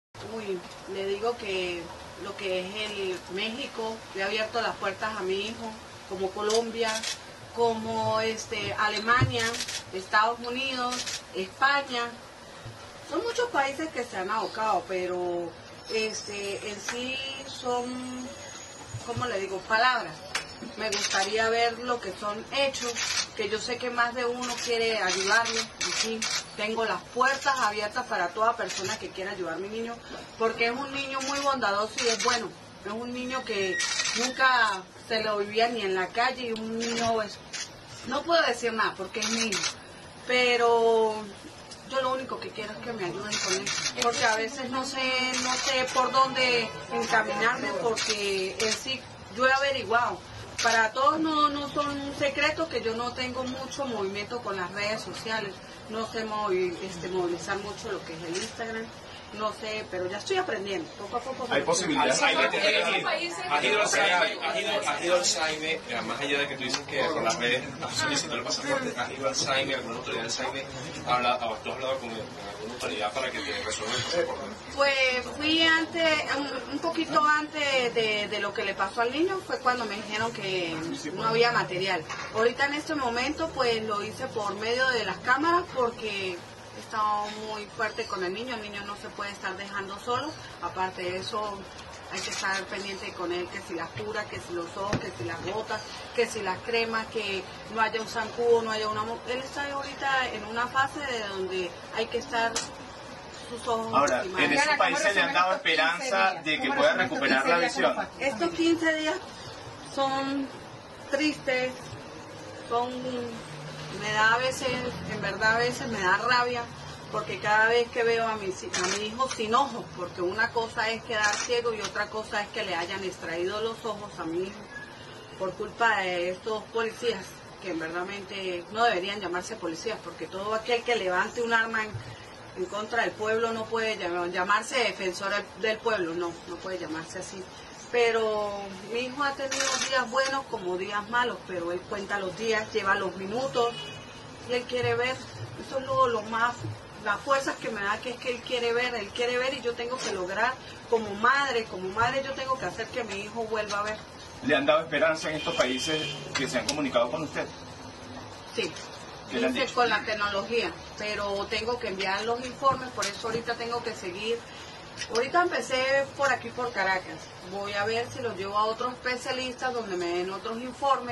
Adriana Parada, madre de Rufo Chacón aseguró este 19Jul que el cambio llegará a Venezuela
Adriana Parada, madre de Rufo Chacón aseguró este 19Jul que el cambio llegará a Venezuela: "Yo se que va a llegar la paz"